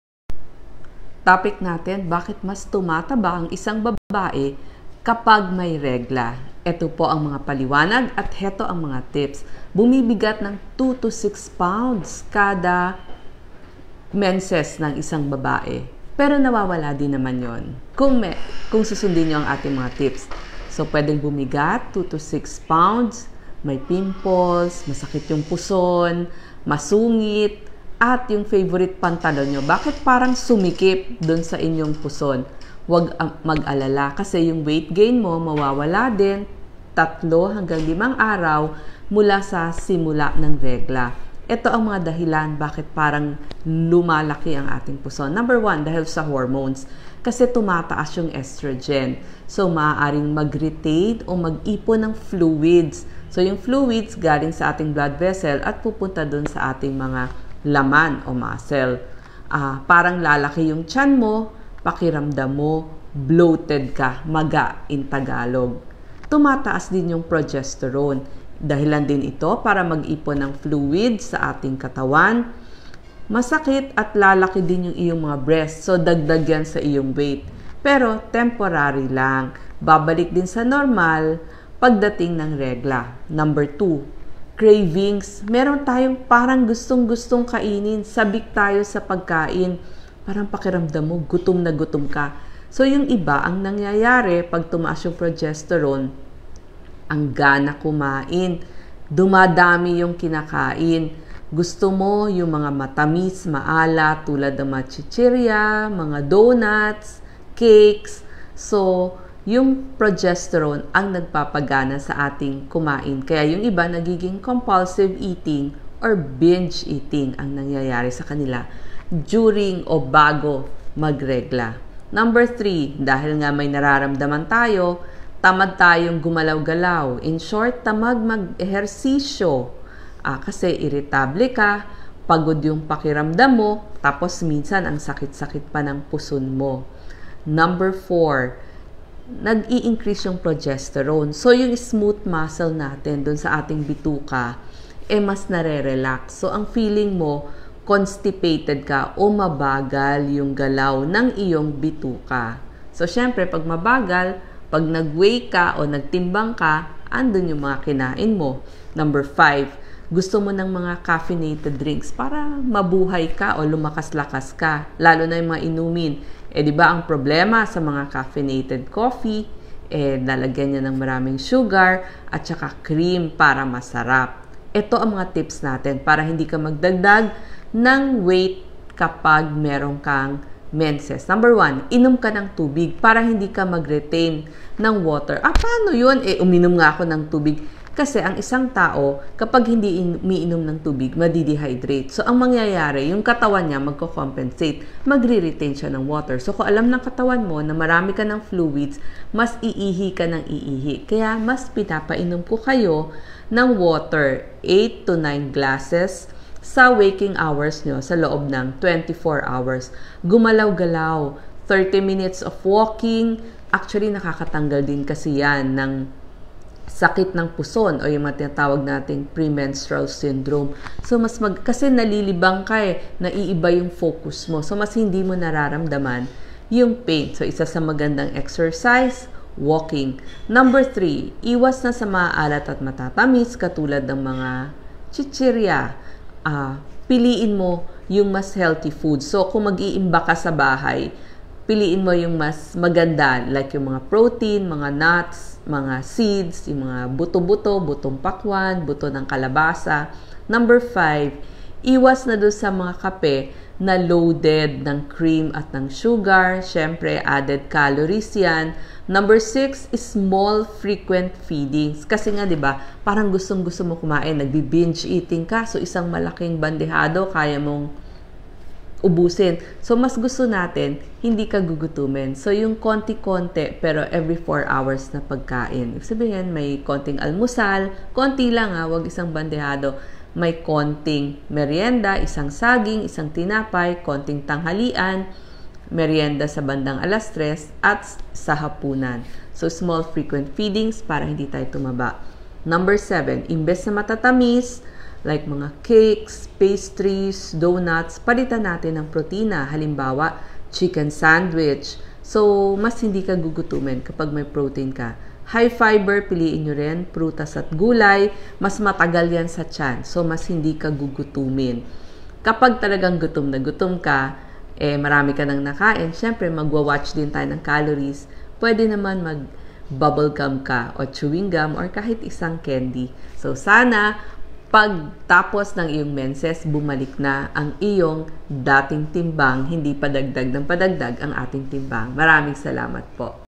Bakit Ako Mataba Kapag May Regla? Paano Paliitin ang Puson. - By Doc Liza Ramoso-Ong
Bakit Ako Mataba Kapag May Regla?
Paano Paliitin ang Puson.
By Doc Liza Ramoso-Ong